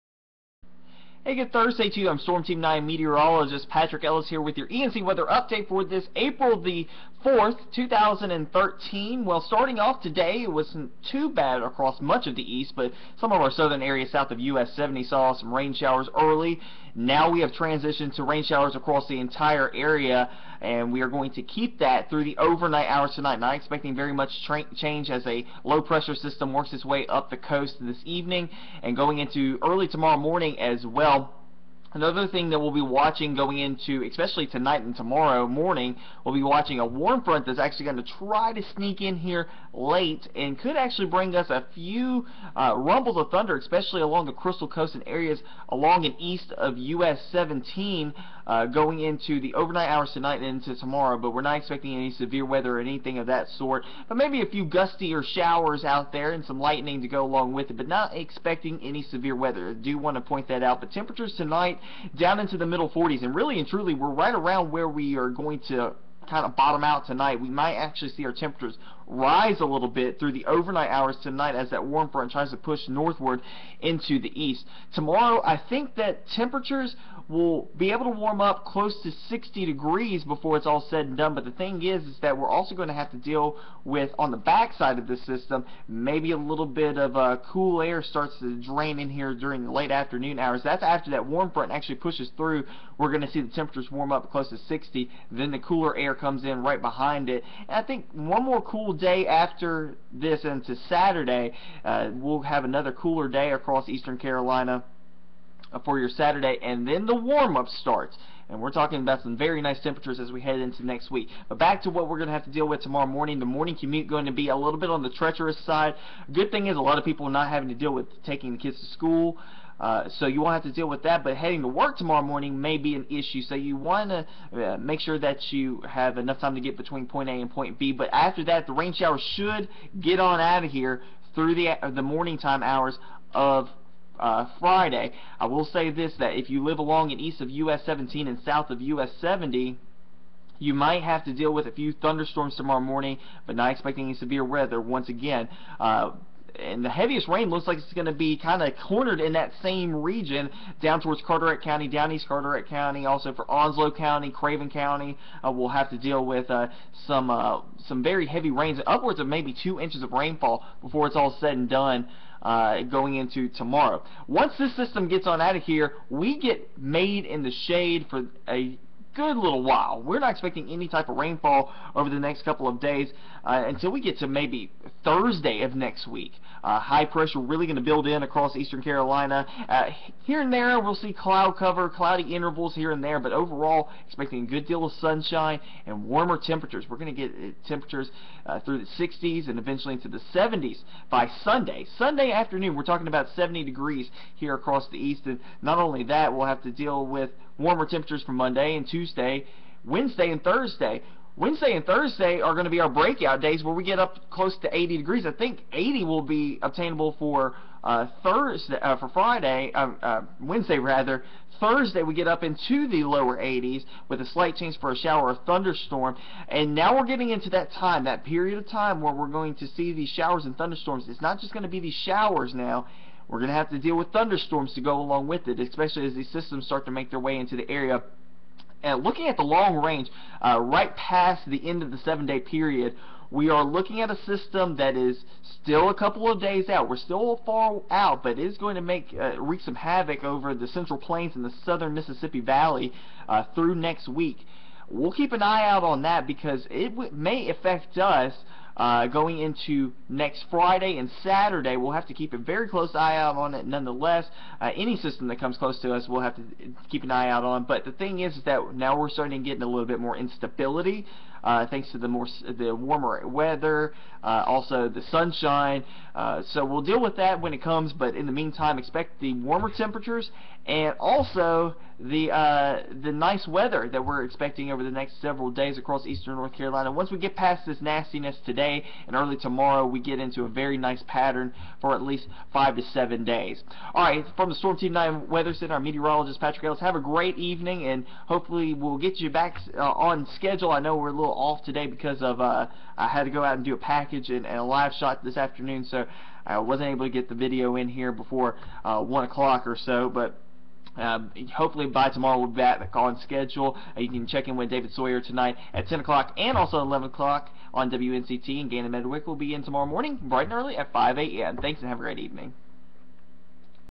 ENC Weather Update 4/4
Storm Team 9 Meteorologist Patrick Ellis talks about a good deal of rain that is expected to fall tonight and early Friday across the East, but nicer and warmer days are in the near future.